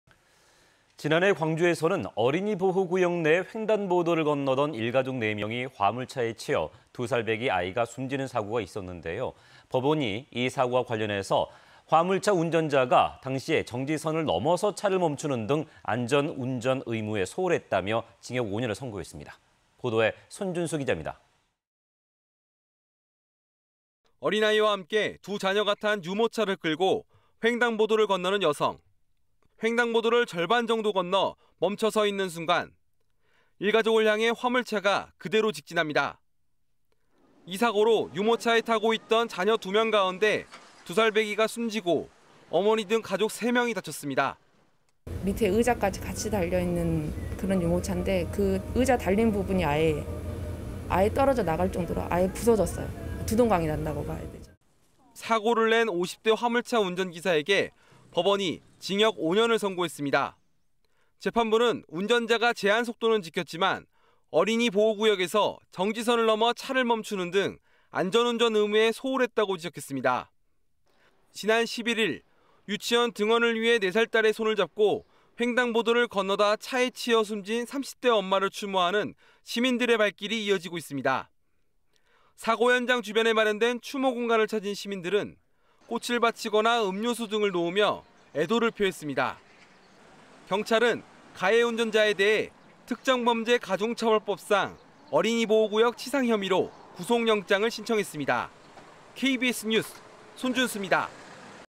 광주 스쿨존 사망사고 5년 선고…인천 사고 현장엔 추모 물결 / KBS 2021.05.14.
[앵커]

 지난해 광주에서 어린이보호구역 내 횡단보도를 건너던 일가족 4명이 화물차에 치여 두 살배기 어린아이가 숨졌는데요.

 법원이 "정지선을 넘어 차를 멈추는 등  안전 운전 의무에 소홀했다"며 화물차 운전자에게 징역 5년을 선고했습니다.

 보도에 손준수 기자입니다.

 [앵커]

 어린아이와 함께 두 자녀가 탄 유모차를 끌고 횡단보도를 건너는 여성.

 횡단보도를 절반 정도 건너  멈춰서 있는 순간, 일가족을 향해 화물차가 그대로 직진합니다.

 이 사고로 유모차에 타고 있던 자녀 2명 가운데 두 살배기가 숨지고  어머니 등 가족 3명이 다쳤습니다.

 [안지숙/인근 상인/지난해 11월 당시 : "밑에 의자까지 같이 달려있는 그런 유모차인데 의자 달린 부분이 아예 떨어져 나갈 정도로 아예 부서졌어요. 두 동강이 났다고 봐야죠."]

 사고를 낸  50대 화물차 운전기사에게  법원이 징역 5년을 선고했습니다.

 재판부는 운전자가 제한속도는 지켰지만 어린이보호구역에서 정지선을 넘어 차를 멈추는 등  안전 운전 의무에 소홀했다고 지적했습니다.

 지난 11일  유치원 등원을 위해  4살 딸의 손을 잡고  횡단보도를 건너다 차에 치여 숨진  30대 엄마를 추모하는  시민들의 발길이 이어지고 있습니다.

 사고 현장 주변에 마련된 추모 공간을 찾은 시민들은  꽃을 바치거나 음료수 등을 놓으며 애도를 표했습니다.

 경찰은  가해 운전자에 대해 특정범죄가중처벌법상 어린이보호구역 치상 혐의로 구속영장을 신청했습니다.

 KBS 뉴스 손준수입니다.

 영상편집:이성훈